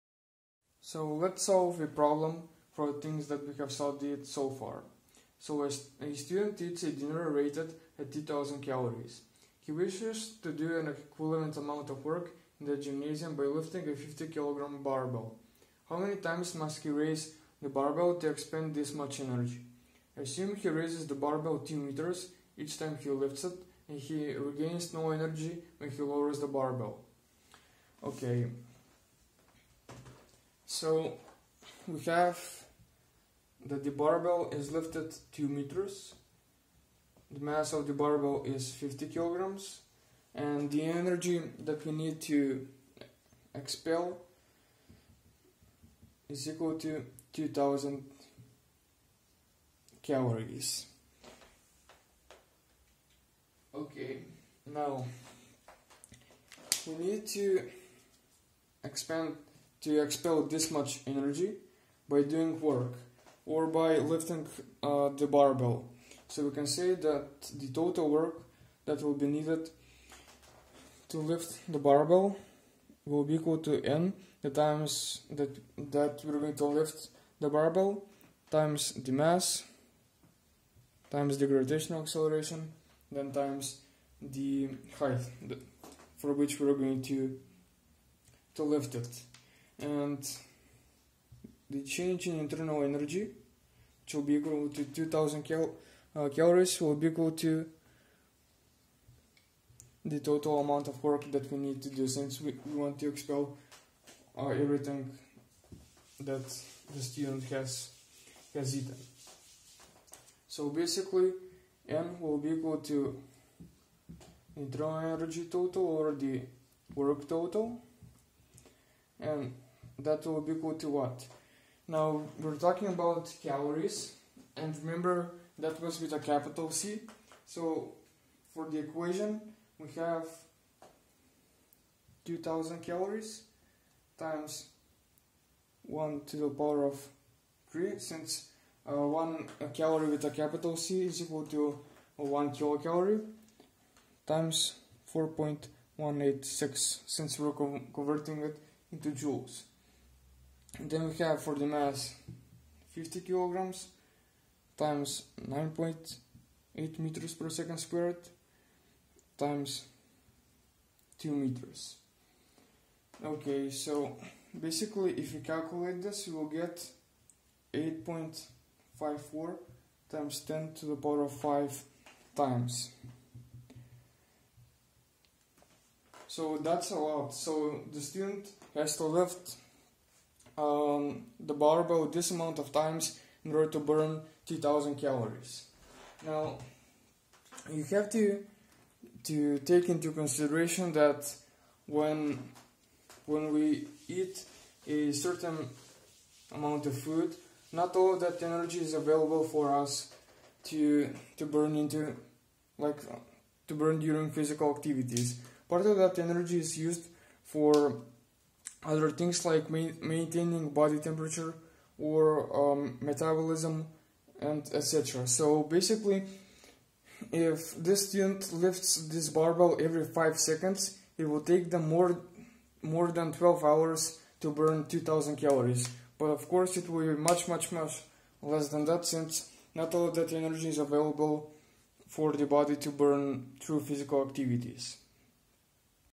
Mechanical equivalence of heat || problem
A student eats a dinner rated at 2 000 Calories. He wishes to do an equivalent amount of work in the gymnasium by lifting a 50.0-kg barbell. How many times must he raise the barbell to expend this much energy? Assume he raises the barbell 2.00 m each time he lifts it and he regains no energy when he lowers the barbell.